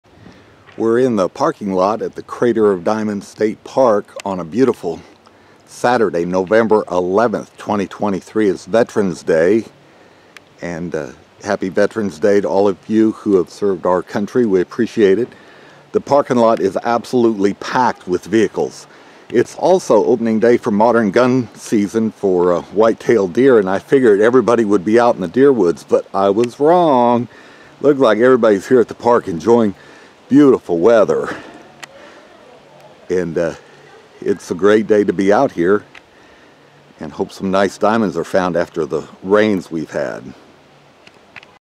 Happy Veterans Day From Arkansas' Crater of Diamonds State Park
It is 64 degrees and sunny here at Southwest Arkansas' Crater of Diamonds State Park.  The parking lot is packed with vehicles today.  Happy Veterans Day and a sincere THANK YOU to all of you who have served our country.